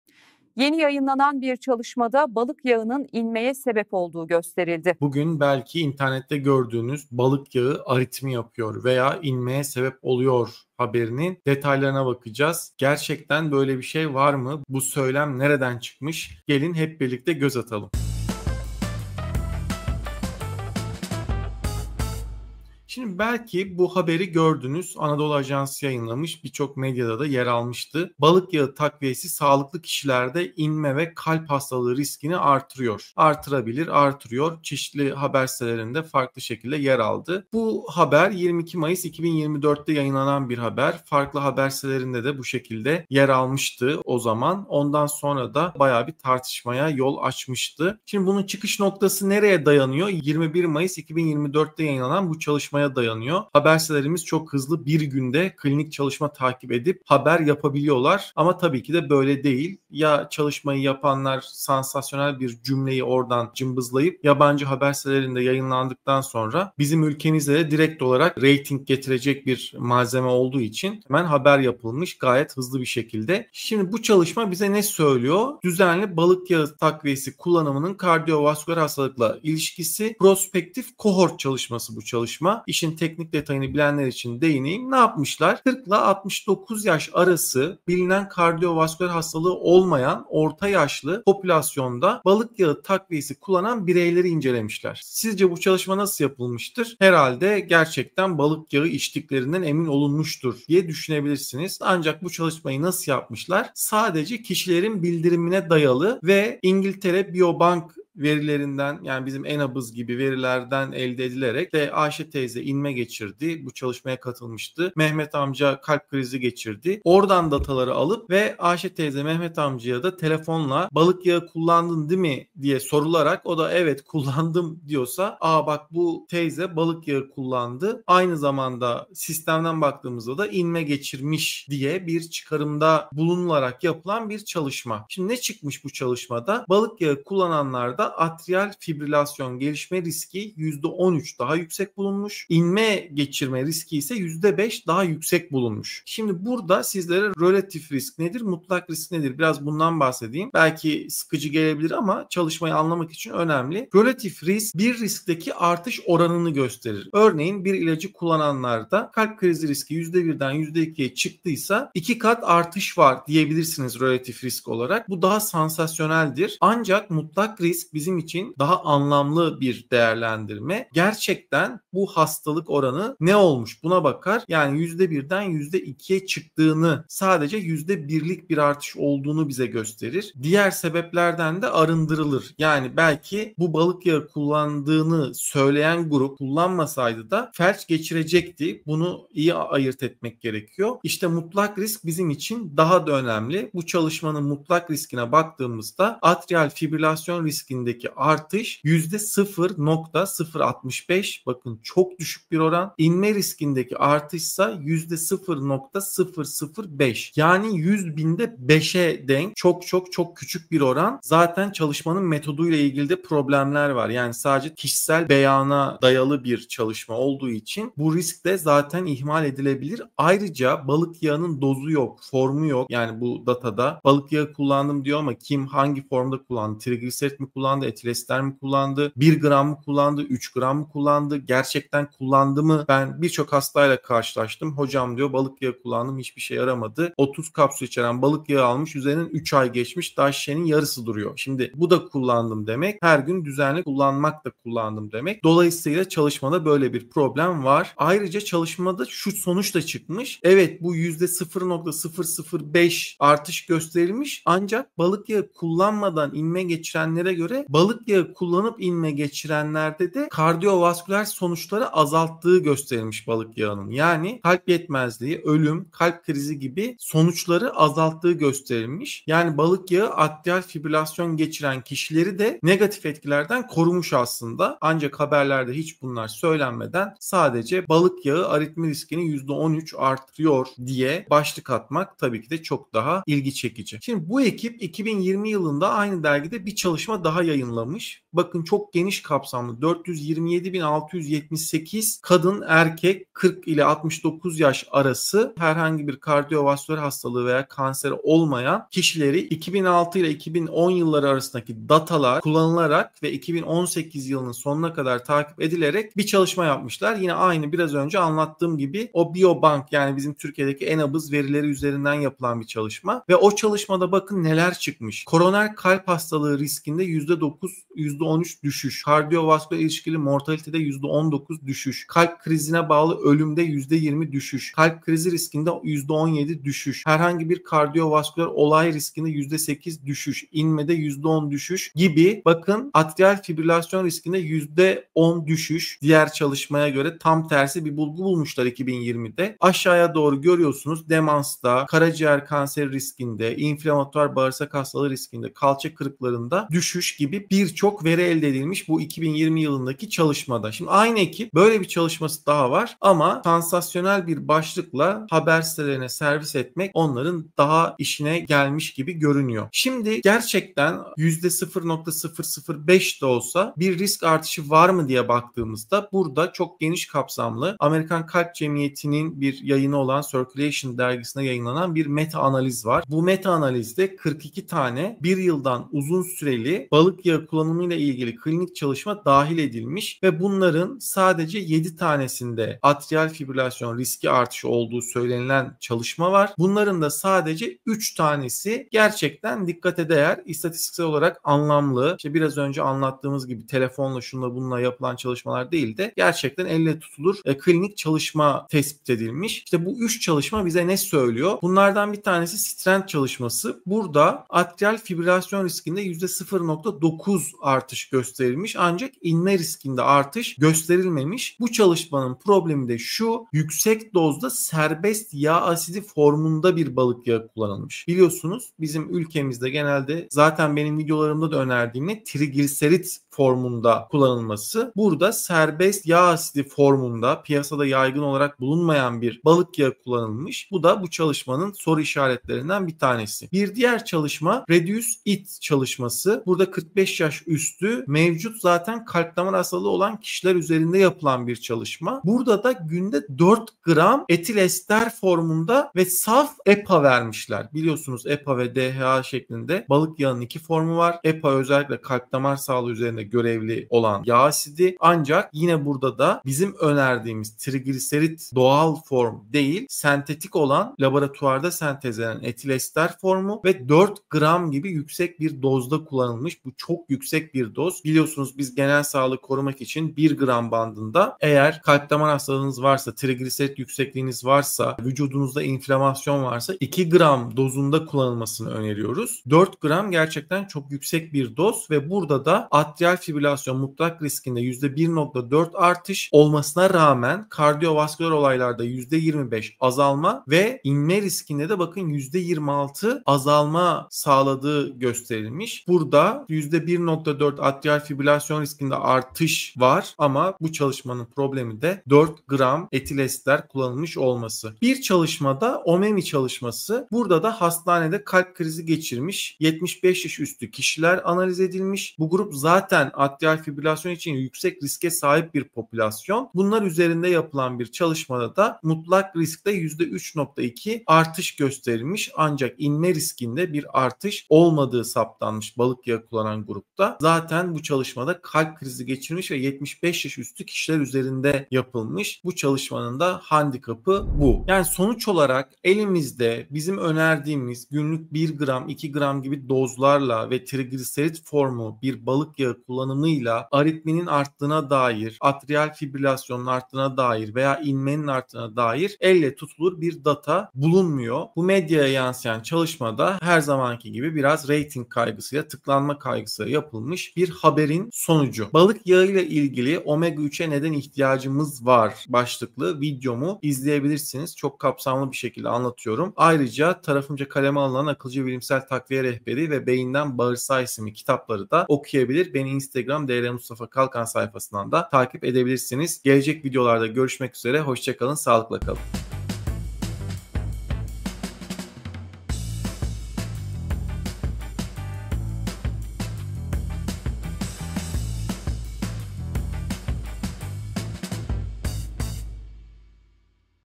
BALIK YAĞI ARİTMİ YAPIYOR MU? (Doğru mu, Yanlış mı?)
"Balık yağı aritmi, atrial fibrilasyon ve inme yapıyor" şeklinde bazı haber sitelerinde yer alan haber doğru mu yanlış mı? Detaylar videoda! Medyanın sansasyonel başlıklarının aksine, balık yağının kalp ve damar sağlığı üzerindeki gerçek etkilerini, yapılan klinik çalışmaların detaylarını ve metodolojik hatalarını öğrenmek için bu videoyu kaçırmayın. Relatif ve mutlak risk kavramlarıyla kafanızdaki soru işaretlerini giderecek, doğru bilgiye ulaşacaksınız.

00:00 Giriş
00:22 Medyada Yer Alan Haberler
00:50 Konunun Çıkış Makalesi
04:43 Aynı Ekibin 2020 Çalışması
06:07 Balık Yağı ve Atrial Fibrilasyon Üzerine Meta Analiz
06:41 Strength Çalışması
07:13 Reduce IT Çalışması
08:22 Omemi Çalışması

Kaynaklar:
Ge Chen, Zhengmin (Min) Qian, Junguo Zhang, Shiyu Zhang, Zilong Zhang, Michael G Vaughn, Hannah E Aaron, Chuangshi Wang, Gregory YH Lip, Hualiang Lin - Regular use of fish oil supplements and course of cardiovascular diseases: prospective cohort study: BMJ Medicine 2024;3:e000451.

Li Z, Zhong W, Liu S, Kraus V B, Zhang Y, Gao X et al. Associations of habitual fish oil supplementation with cardiovascular outcomes and all cause mortality: evidence from a large population based cohort study BMJ 2020; 368 :m456 doi:10.1136/bmj.m456

Gencer B, Djousse L, Al-Ramady OT, Cook NR, Manson JE, Albert CM. Effect of Long-Term Marine ɷ-3 Fatty Acids Supplementation on the Risk of Atrial Fibrillation in Randomized Controlled Trials of Cardiovascular Outcomes: A Systematic Review and Meta-Analysis. Circulation. 2021 Dec 21;144(25):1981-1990. doi: 10.1161/CIRCULATIONAHA.121.055654. Epub 2021 Oct 6. PMID: 34612056; PMCID: PMC9109217.

Nicholls SJ, Lincoff AM, Garcia M, et al. Effect of High-Dose Omega-3 Fatty Acids vs Corn Oil on Major Adverse Cardiovascular Events in Patients at High Cardiovascular Risk: The STRENGTH Randomized Clinical Trial. JAMA. 2020;324(22):2268–2280. doi:10.1001/jama.2020.22258

Dr. Mustafa Kalkan Youtube kanalımda, gıda takviyeleri ve sağlıklı yaşam ile ilgili güncel ve bilimsel gelişmeleri aktarıyorum.

► Bu videoda neler var?
sorularının cevabını bulabilirsiniz.